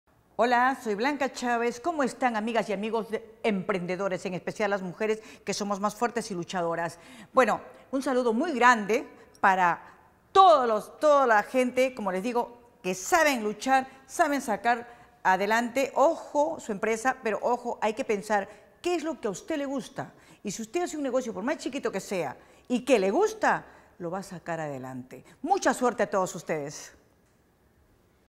Somos Empresa | Blanca Chávez
Blanca Chavez, empresaria, dueña del Restaurant El Rocoto, saluda a los fans de Somos Empresa.
Si quieres emprender o necesitas de orientación para mejorar tu emprendimiento visita